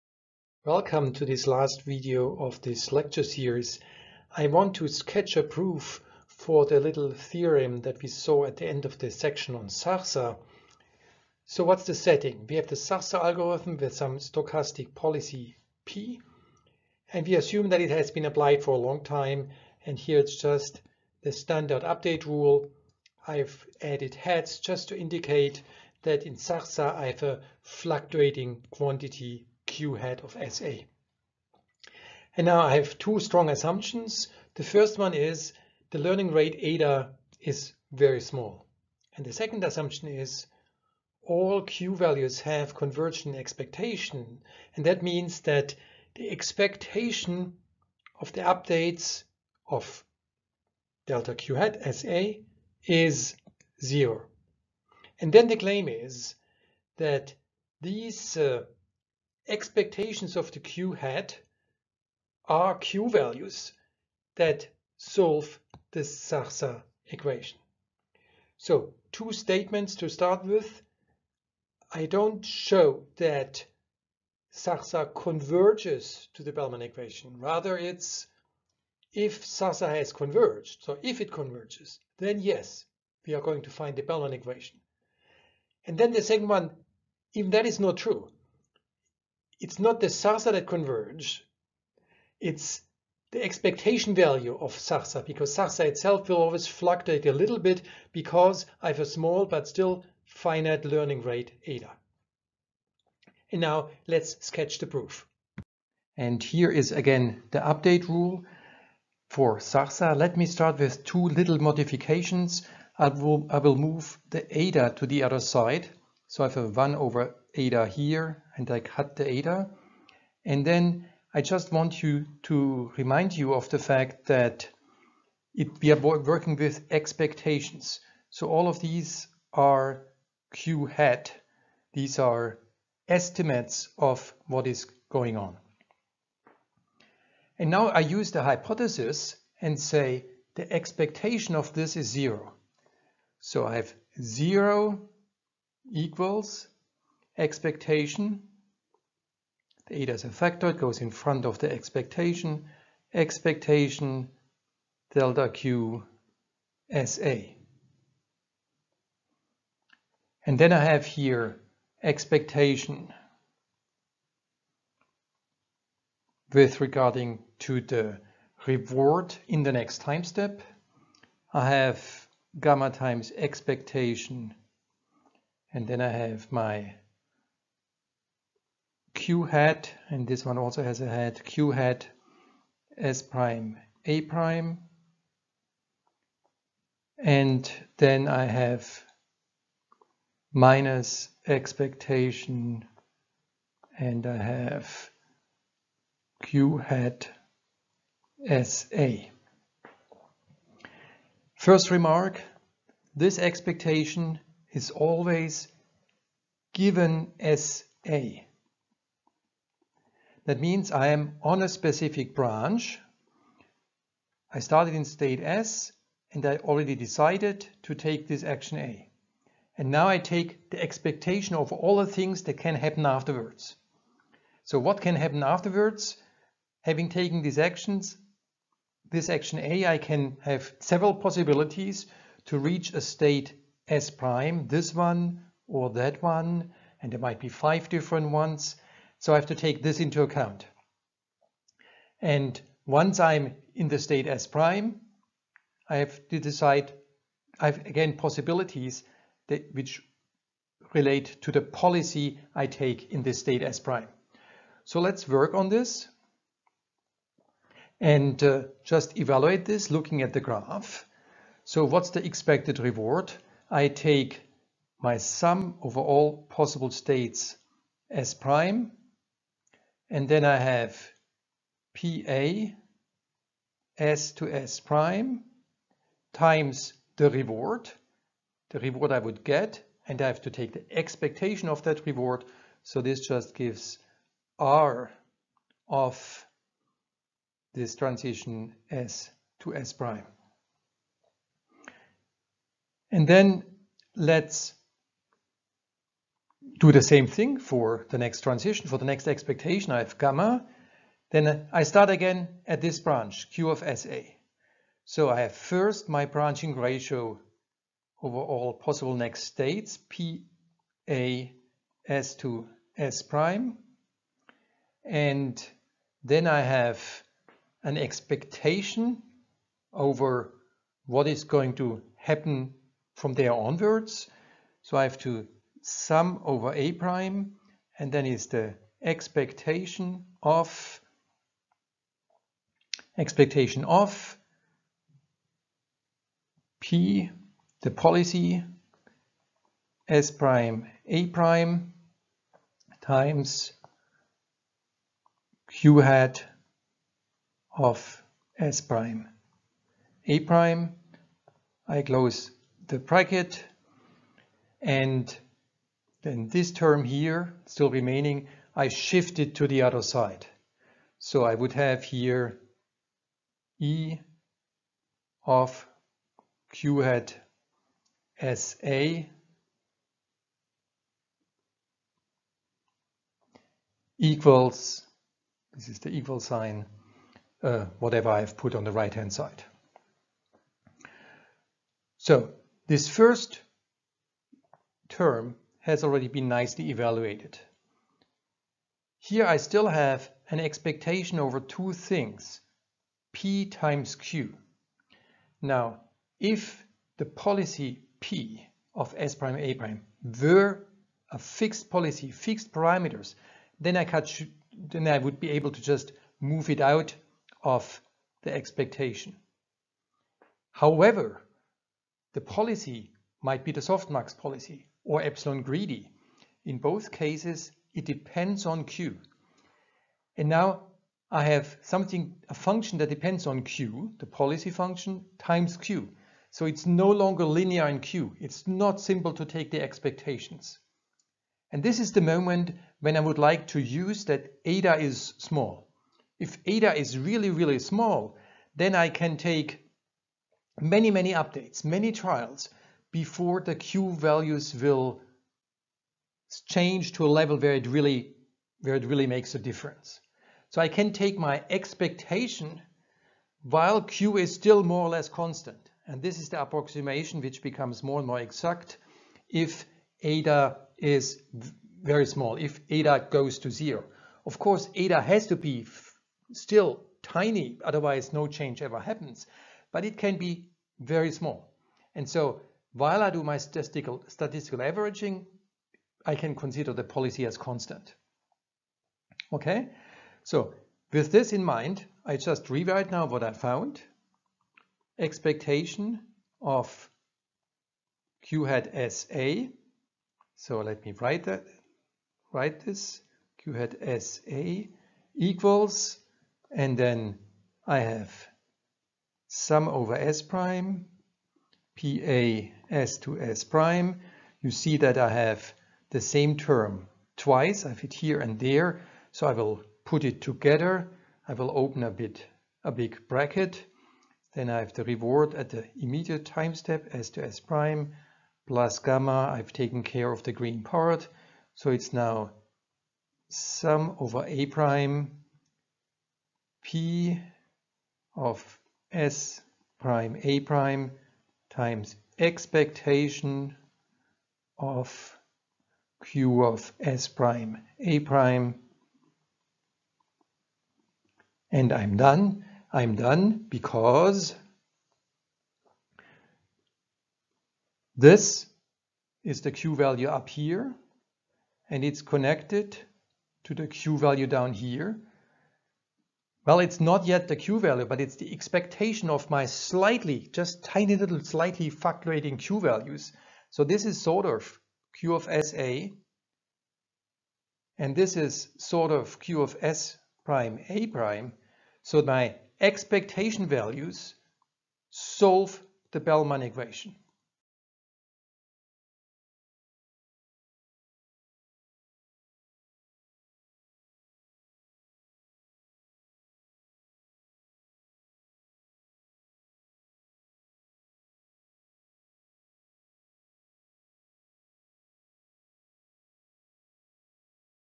RL1.6B Proof Sketch: SARSA is consistent with Bellman equation
Online SARSA updates cause fluctuations of the estimated Q-values. Here we show that under some conditions the temporal mean of the Q-values solve the Bellman equation